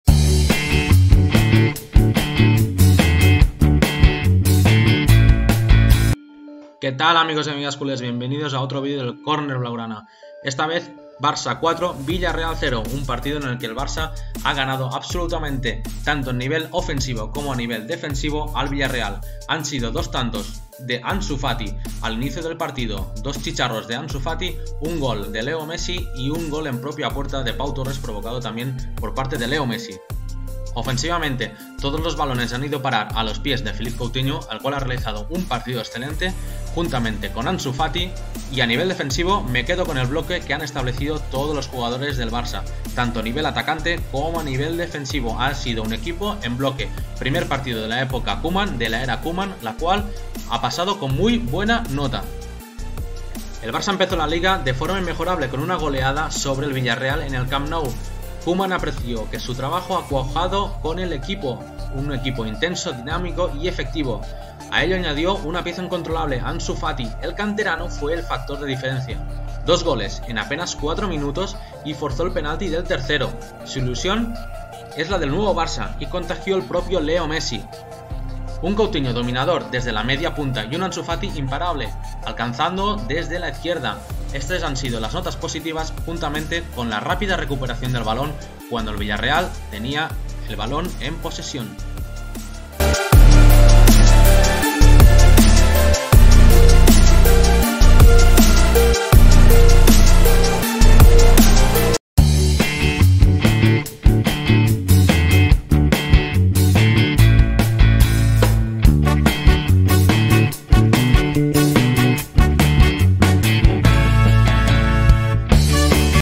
🚨RESUMEN➡️ BARCELONA 4 - 0 VILLARREAL (LA OTRA CARA)
¡Qué tal amig@s culés! El primer partido de la era Koeman acaba con 4 - 0! Doblete de Ansufati, Coutinho estelar y porteria a 0!!